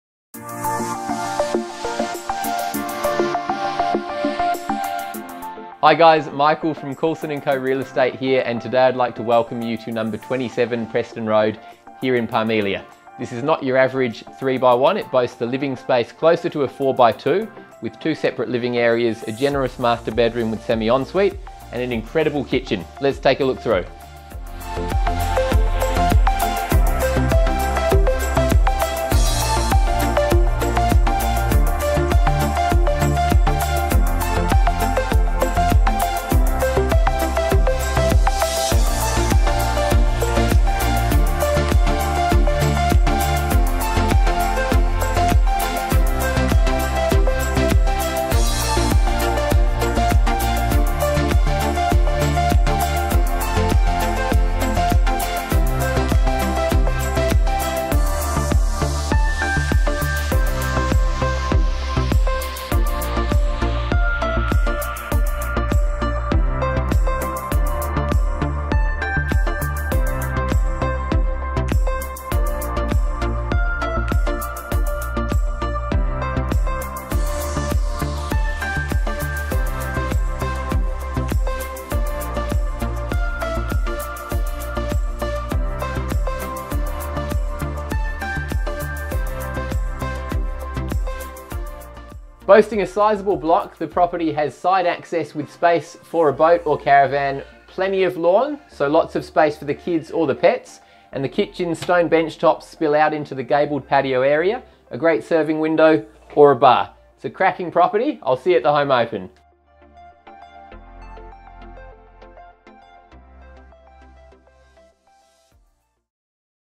27 Preston Rd
Team Coulson from Coulson & Co Real Estate are proud to present 71a Perham Crescent, Leda to the market.